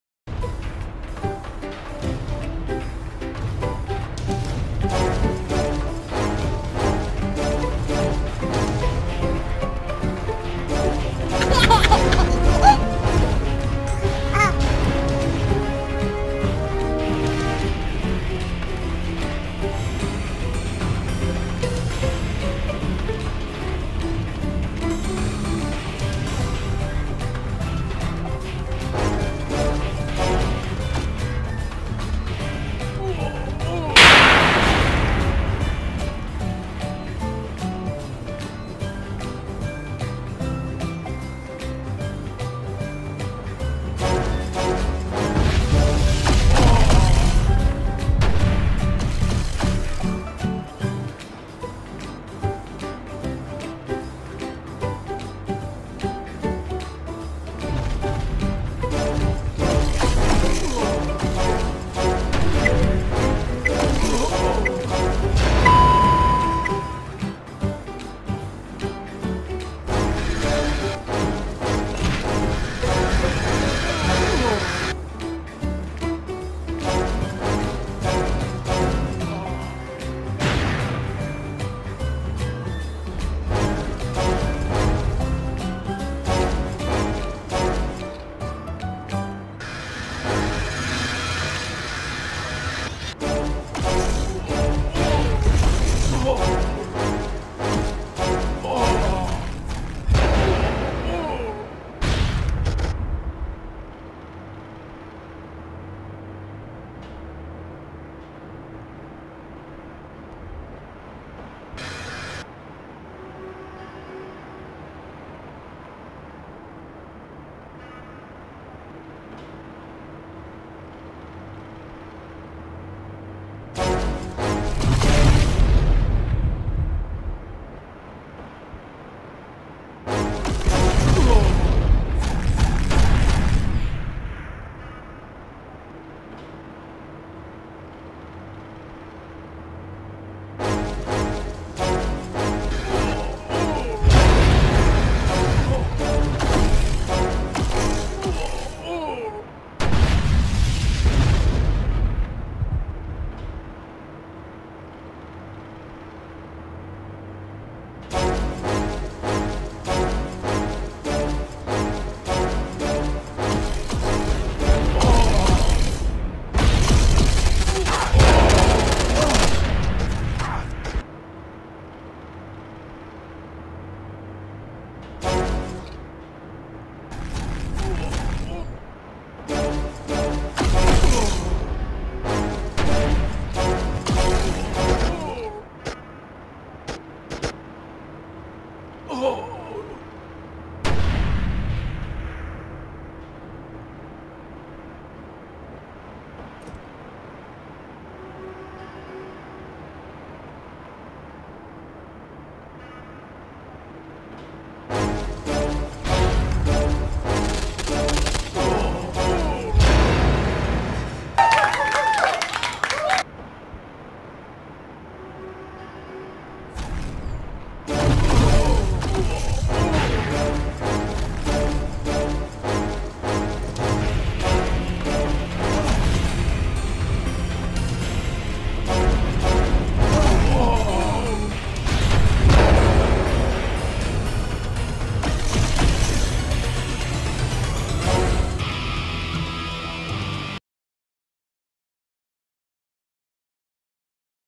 Trolling people in the new event
Killing people while they do there event